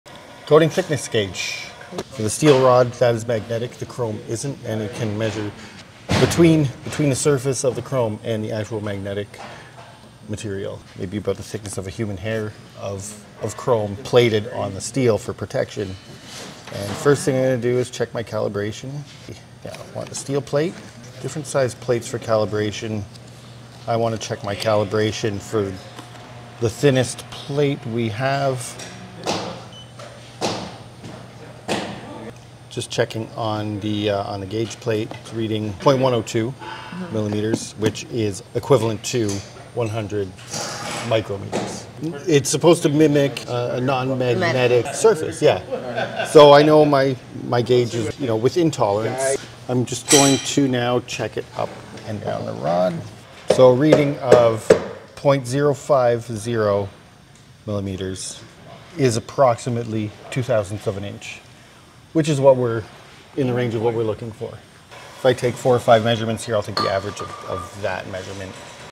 Measuring Chrome Thickness on Steel Rod
Using a coating thickness gauge, we check the chrome plating on a magnetic steel rod on this cylinder—aiming for about 20,000ths of an inch. Calibration is confirmed first, then multiple readings are taken along the rod to ensure consistent protective coating.